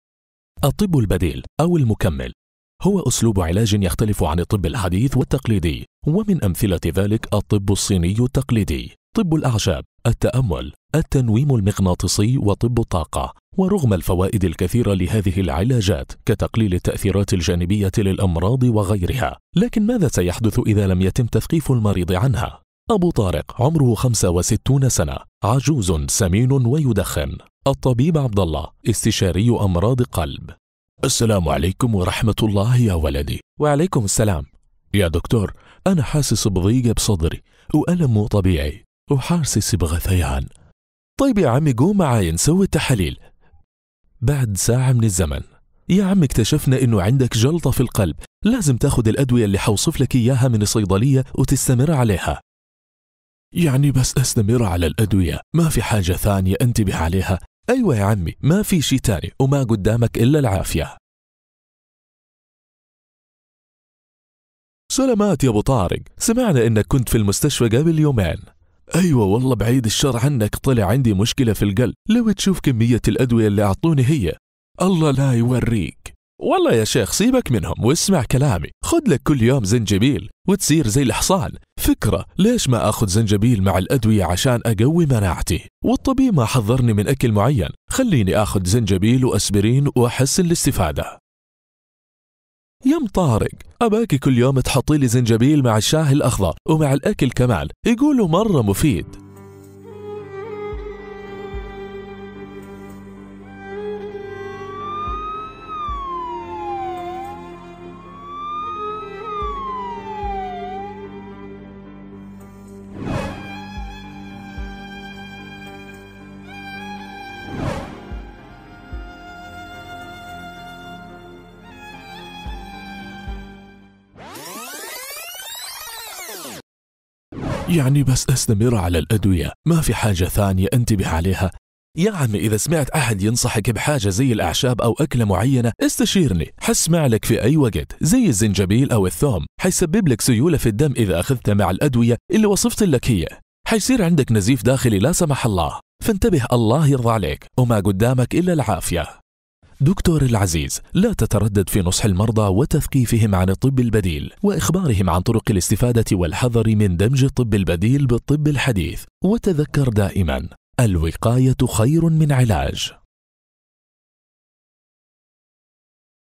الطب البديل
موشن جرافيك الطب البديل او المكمل كأسلوب علاجي يختلف عن الطب الحديث